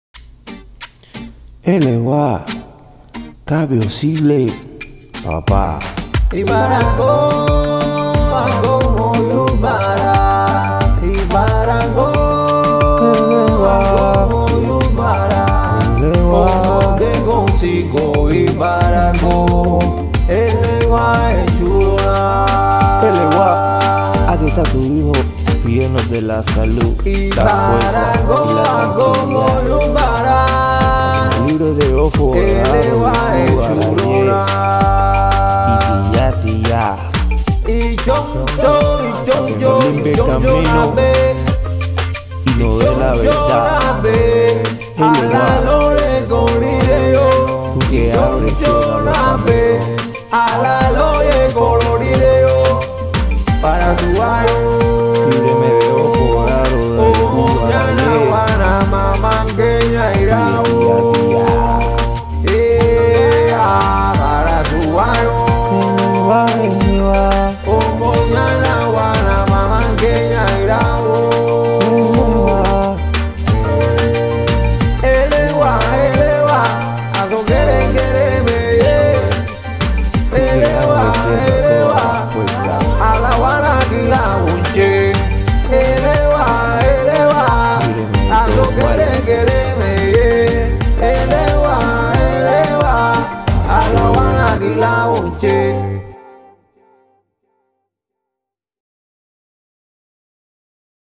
My Movie 14 Canto pa ELEGUA
Esto es una experimentación de ritmo con canto afro cubano para el santo ELEGUA  de Acokan  Juan Del Risco